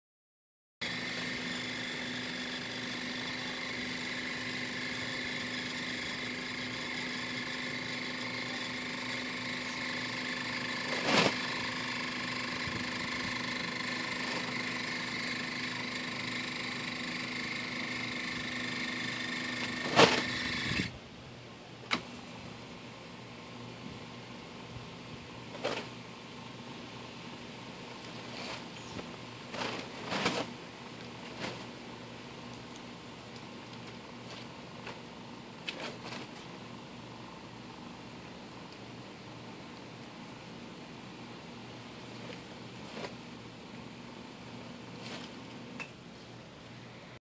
Chicken coop door closer
Linear actuator rigged up to open and close the door on our chicken coop. Still need to automate it. This device runs off of a solar charged lead acid battery. No AC required.